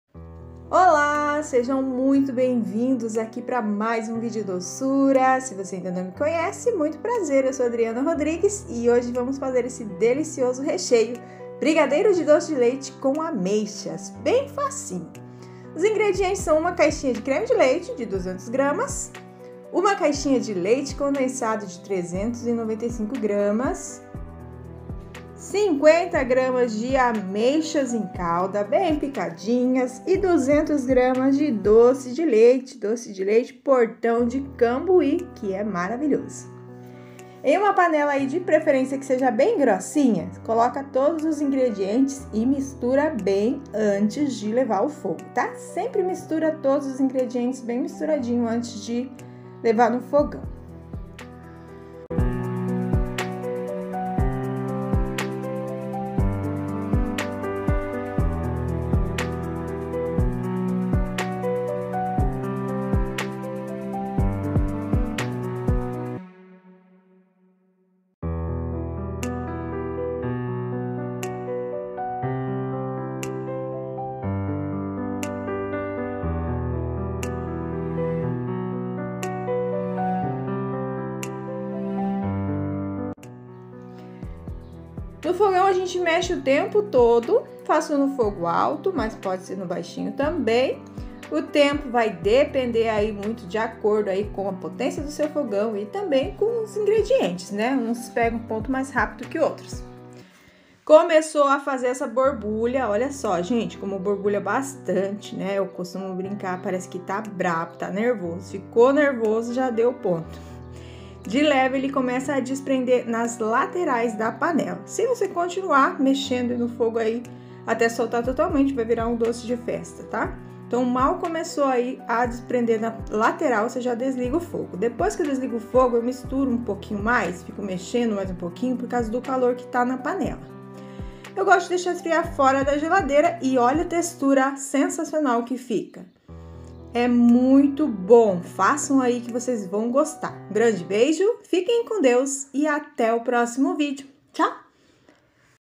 TRADICIONAL RECHEIO DE DOCE DE LEITE COM AMEIXA
Ingredientes:
395 gramas de leite condensado
200 gramas de creme de leite
200 gramas de doce de leite
50 gramas de ameixa em calda picadinhas